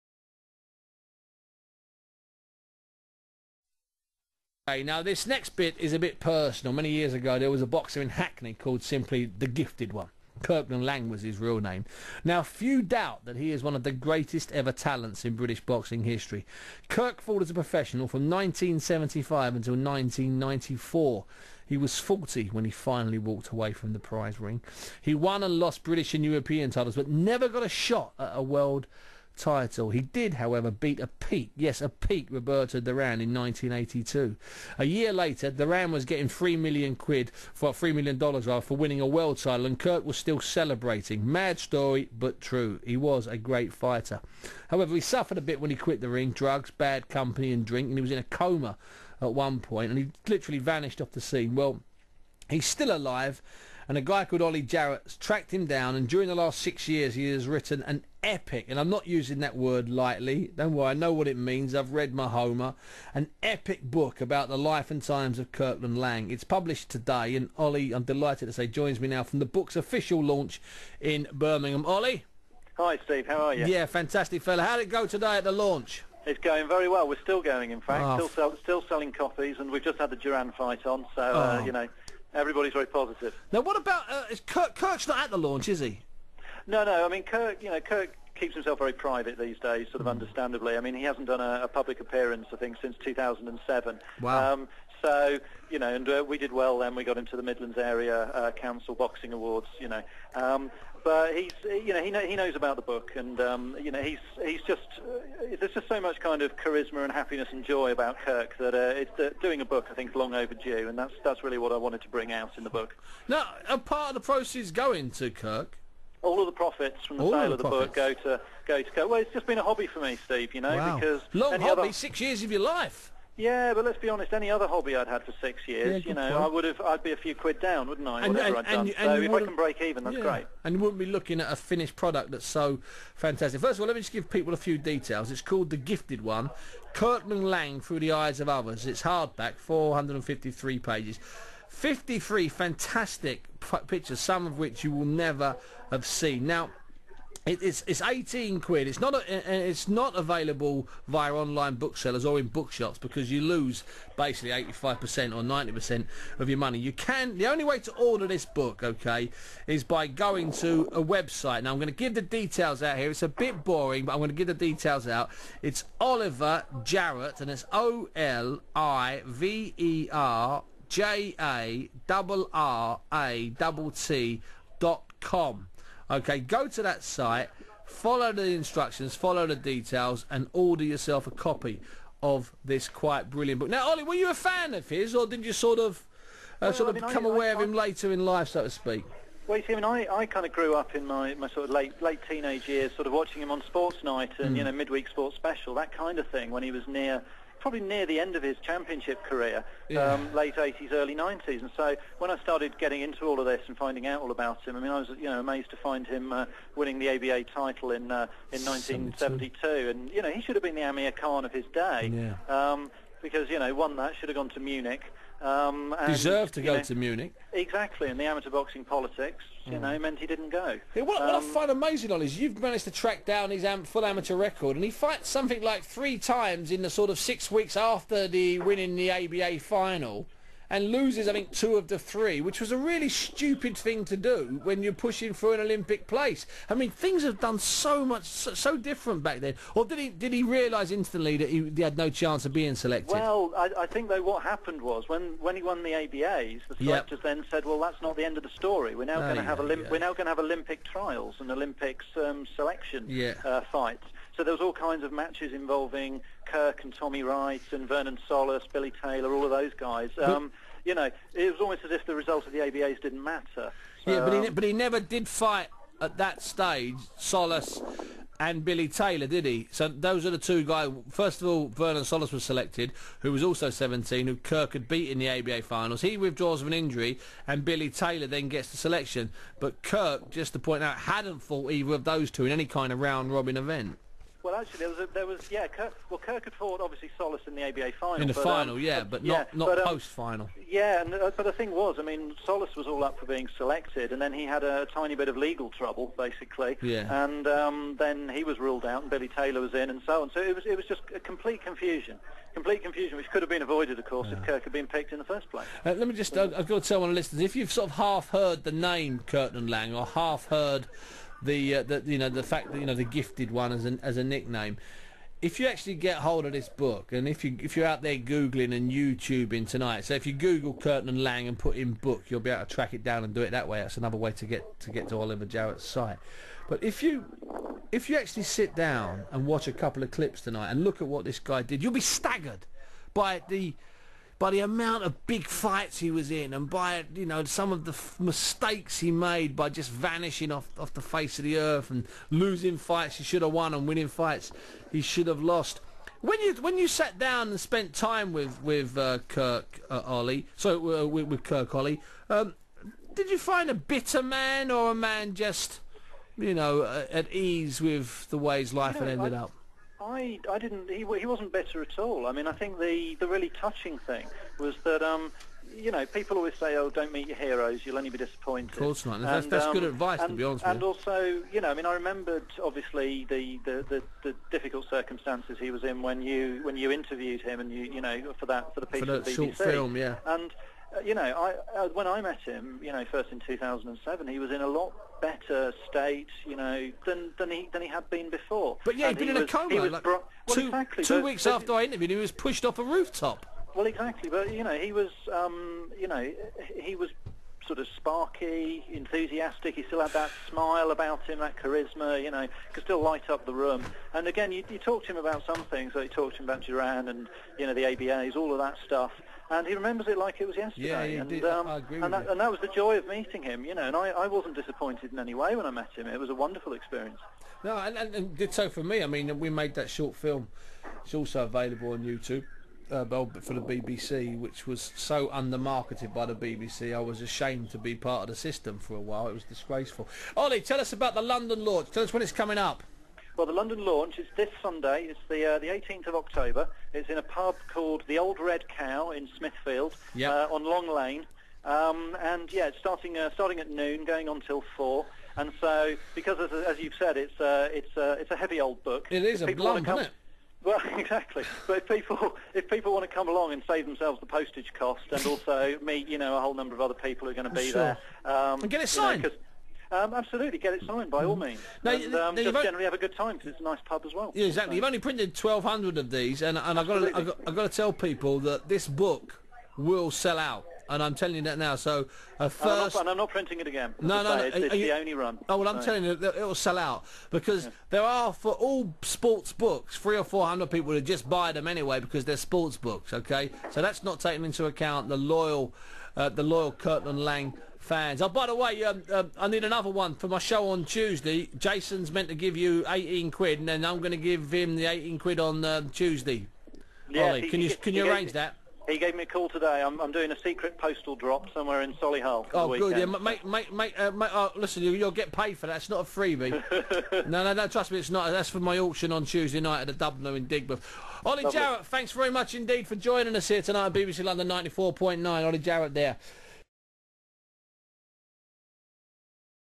Oliver Jarrett On New Kirkland Laing Book
Interview with Oliver Jarrett who has written a new book on the career of Kirkland Laing called 'The Gifted One'